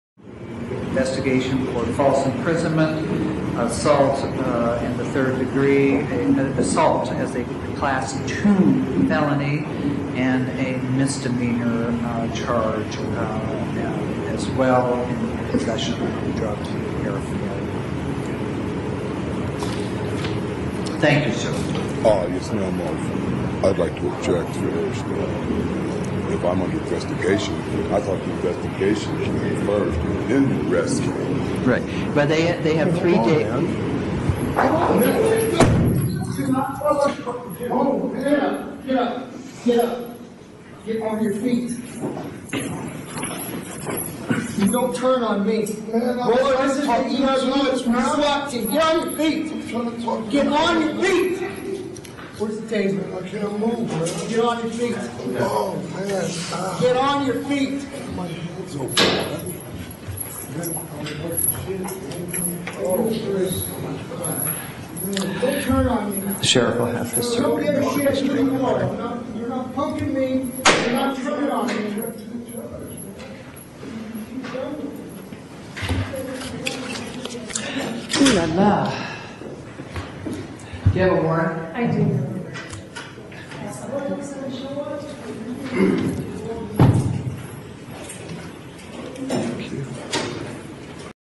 Courtroom video shows Denver sheriffs deputy attacking shackled inmate
DENVER — Denver's safety department waited a year to discipline a sheriff's deputy for grabbing a man who was appearing before a judge in a Denver County courtroom and slamming him into a wall.

Read the full story by Susan Greene at The Colorado Independent,